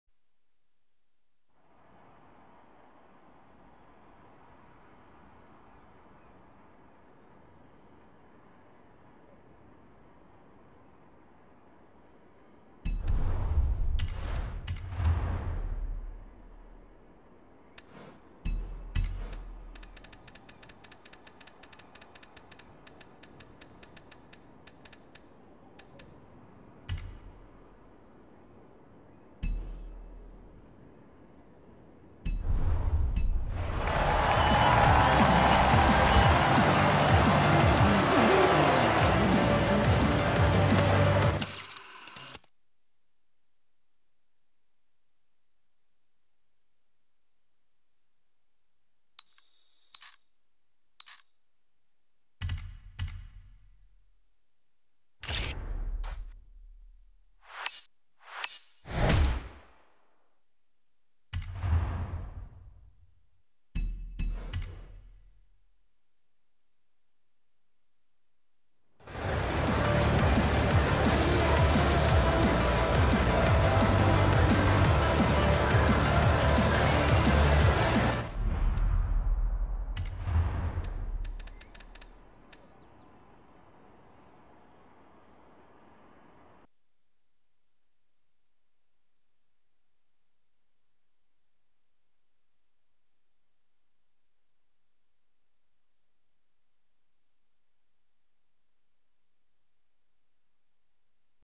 DÉFI PHOTO
Photographiez un JOUJOU DE CIRCUIT devant la scène principale de l'antenne HORIZON RUSH à l'EST de la carte !